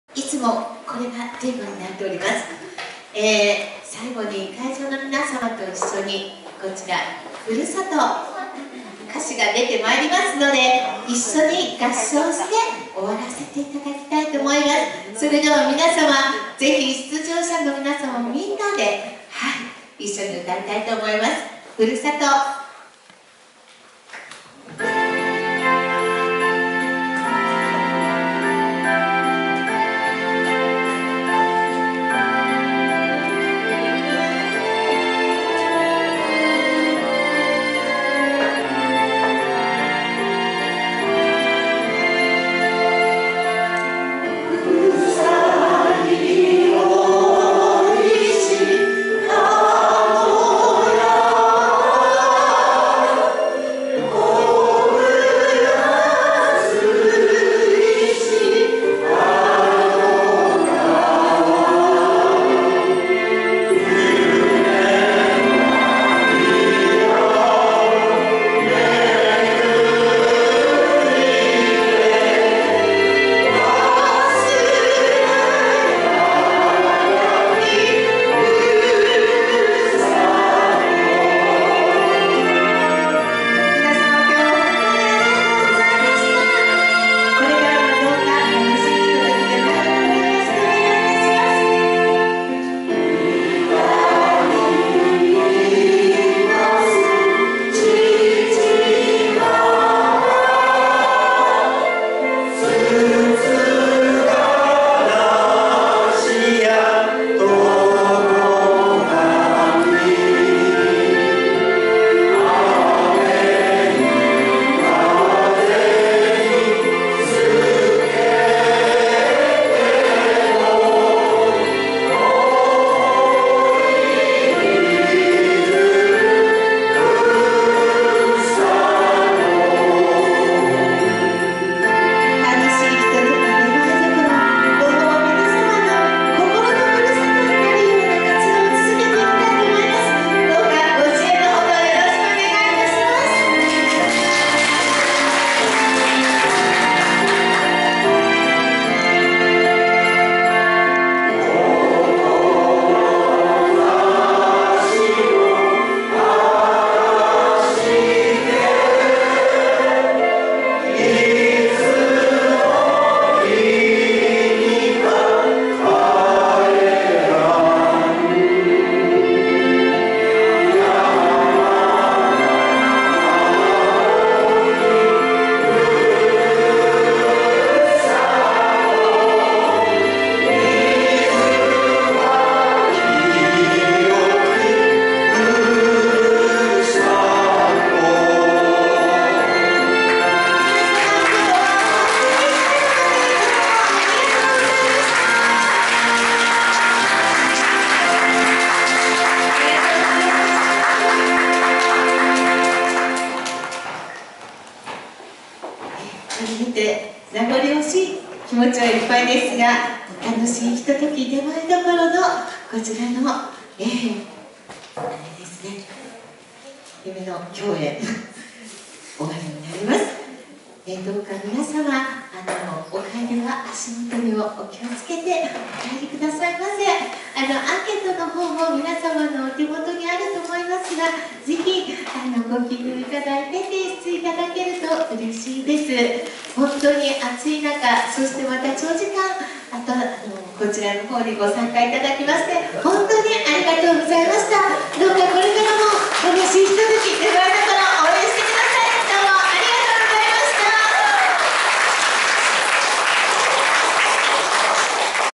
9月7日の午後、中目黒ＧＴプラザホールで実施されたボランティアグループ「楽しいひととき出前どころ」の《祭り》の様子佐野さんと全員。
全員の合唱と演奏